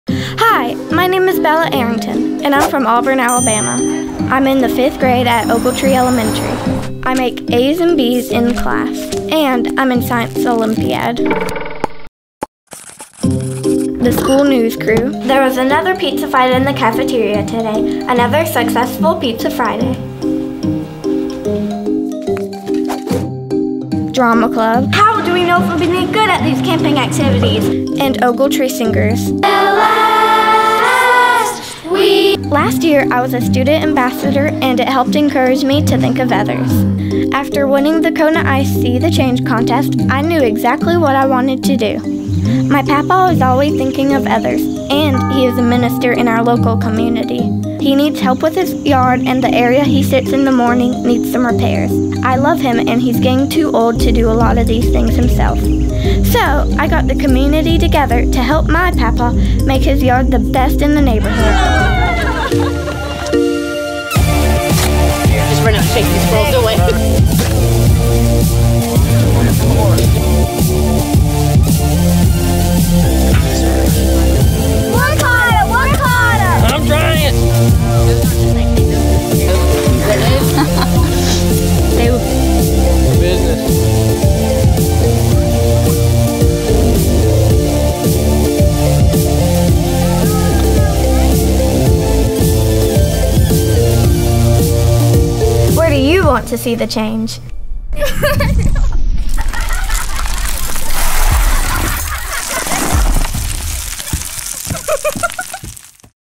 SEEtheCHANGE | Alabama
Bella Arrington, a 5th grader in Auburn, Alabama, wanted to see the change with her great grandfather, Carlton. Watch the video to see how she made a difference in her community.